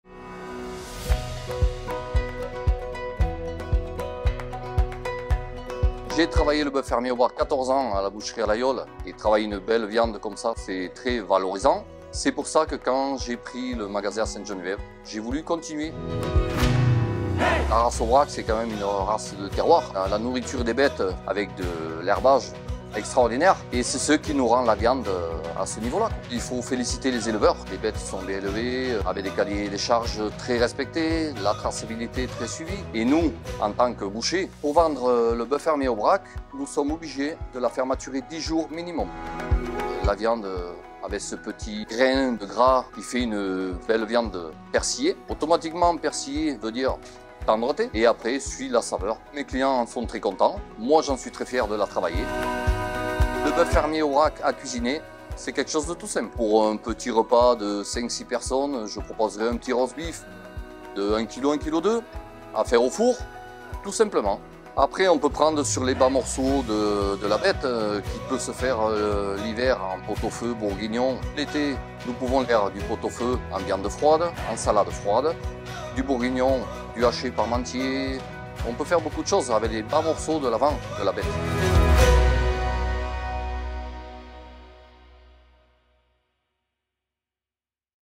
Boeuf Fermier Aubrac - David, Boucherie Maison Conquet Ste Geneviève sur Argence
Découvrez David, notre boucher au magasin de Sainte Geneviève qui vous propose tous les jours le meilleur du Bœuf Fermier Aubrac Label rouge. 🥩

Nous sommes fiers de vous servir et mettre en valeur cette magnifique viande persillée, pleine de saveurs. Grâce à notre savoir-faire de bouchers, nous découpons et mettons en valeur tous les morceaux de la bête de façon artisanale et professionnelle. 🔪

Merci à tous nos éleveurs partenaires qui élèvent et chérissent leurs vaches tout au long de l’année. C’est grâce à eux que vous pouvez vous régaler avec du Bœuf Fermier Aubrac. 🐮

La dernière étape pour sublimer cette viande d’exception est dans vos mains ! Alors n’hésitez pas à demander à nos bouchers des conseils cuisson, nous serons ravis de vous renseigner pour le plus grand bonheur de vos papilles… 😋

📷 Réalisation : Boeuf Fermier Aubrac - Balsa Prod